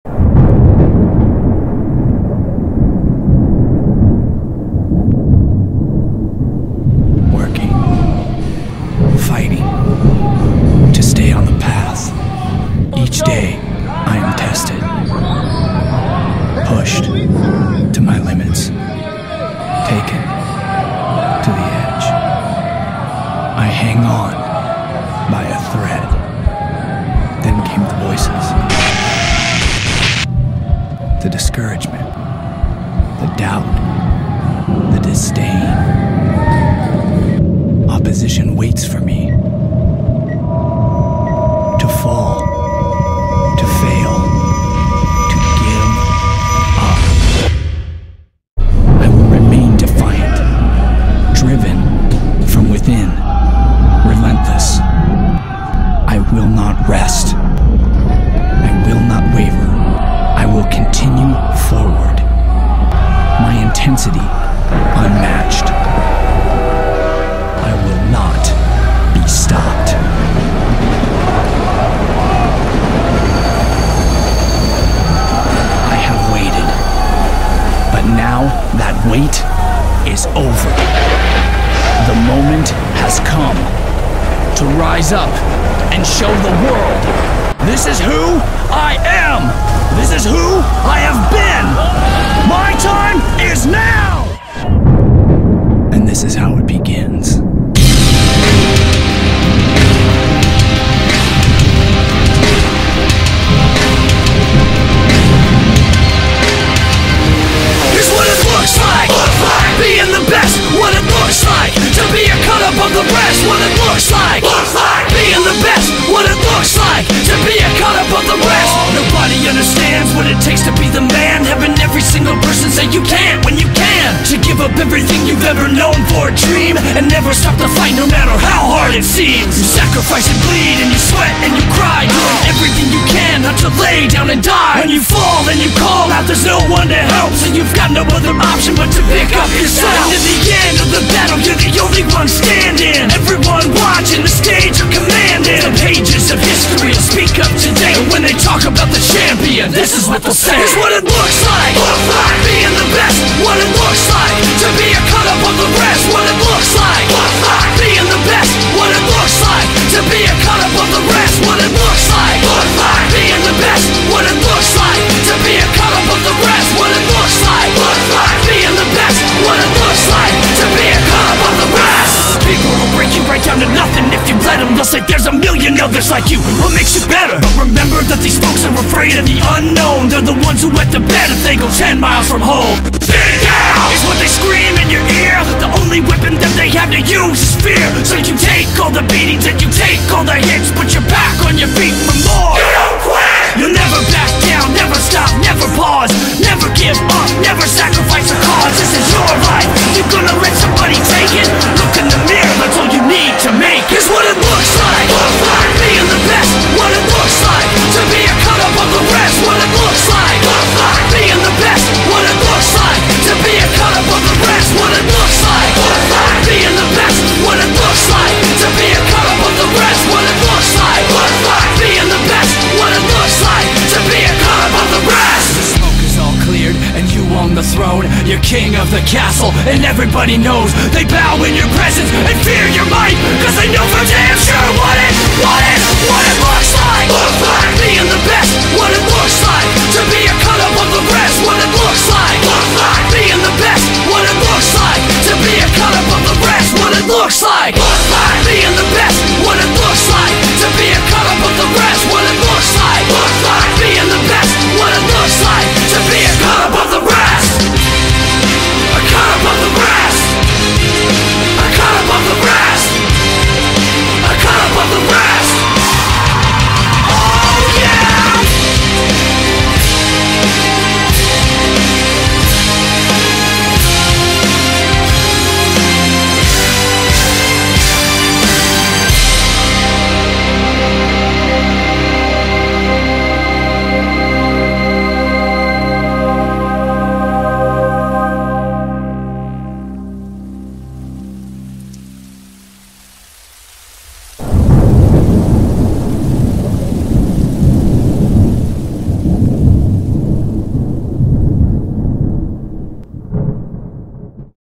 A cut above football hype vid 2024 summer pre season
High School varsity football hype video
Fall Creek Crickets
Fall Creek High School, Fall Creek, WI
Ron Schultz Field
Head Coach Marcus Wahleithner
Cloverbelt High School Football Conference - Fall Creek, Mondovi, Regis, Stanley-Boyd, Elk Mound, Durand, Neillsville-Grantin, Osseo